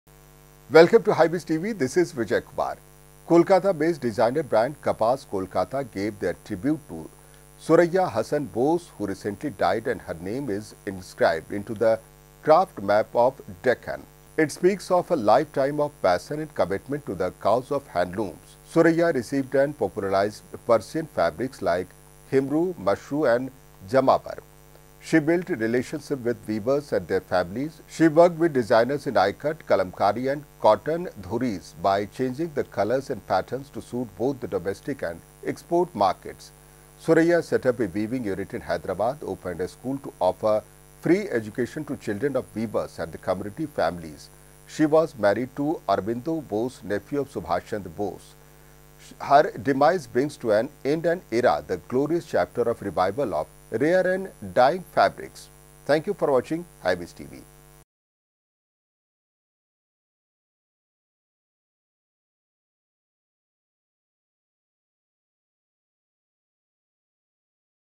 Tribute to Suraiya Hasan Bose Textile Revivalist | Hybiz tv
Kolkata-based designers Brand Kapaas Kolkata gave their tribute to Suraiya Hasan Bose, who recently died and her name is inscribed into the Craft map of the Deccan. It speaks of a life time of Passion & Commitment to the cause of Handlooms.

Suraiya received and popularized Persian Fabrics like Himroo, Mashroo and Jamawar. She built relationships with weavers and their families. She worked with Designs in IKAT, KALAMKARI, and COTTON DHURRIES by changing the colors and patterns to suit both the Domestic & Export Markets.

Suraiya set up a weaving unit in Hyderabad. Opened a school to offer free education to the children of weavers and the community families. She was married to Aurobindo Bose nephew of Subash Chandra Bose. Her demise brings to an end an ERA the Glorious Chapter of Revival of Rare & Dying Fabrics.